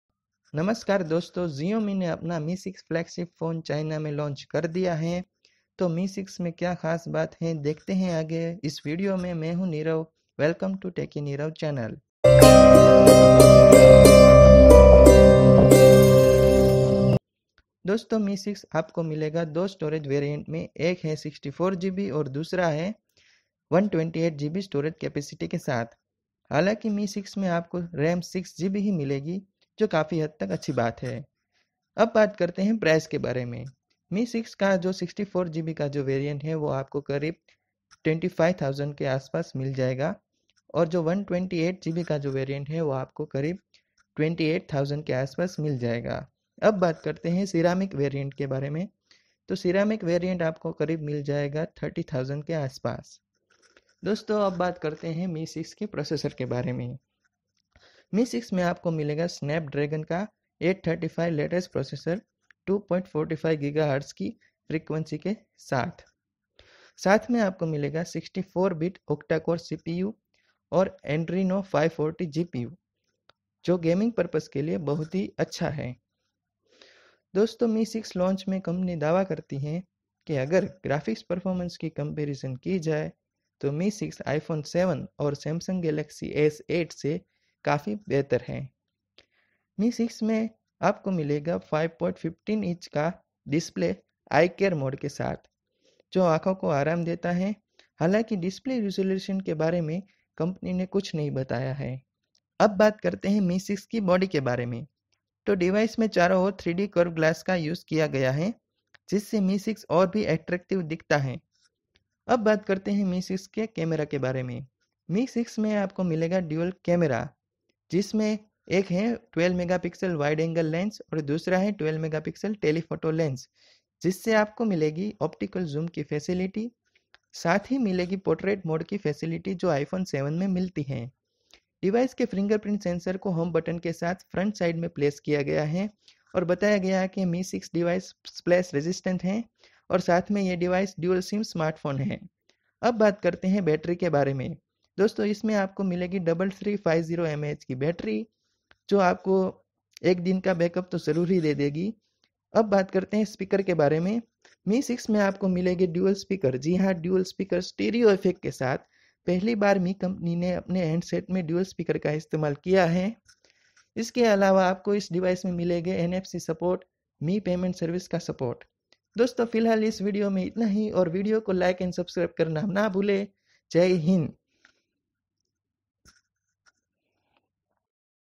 Xiaomi Mi6 Flagship Lounch Specification and Review
Namaskar dosto Xiaomi ne MI6 launch kar diya hai Snapdragon 835 processor ke sath apko milegi 6gb ram 64gb and 128gb varient ke sath.